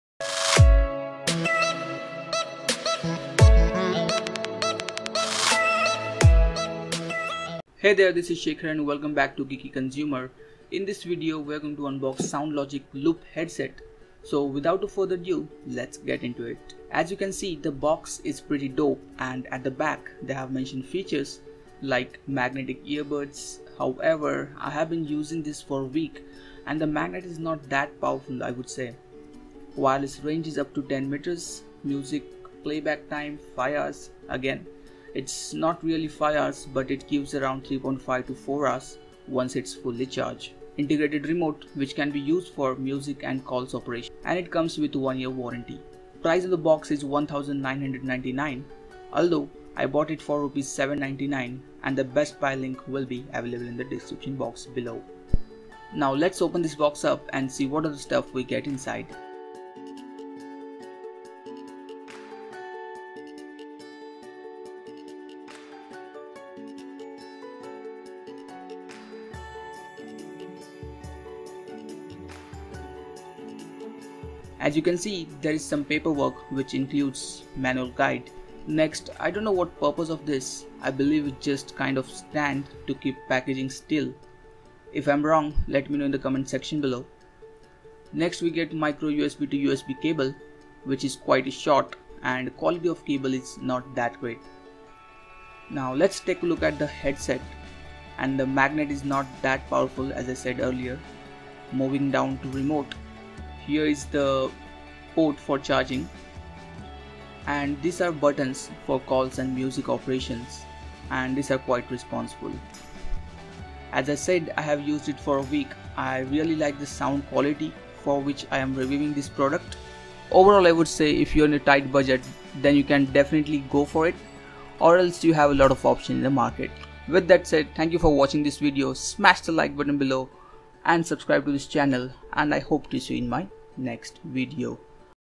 SoundLogic Loop Headset Bluetooth Headset with Mic
Quick unboxing video of SoundLogic Loop Headset

Best Buy Link(SoundLogic Loop Headset)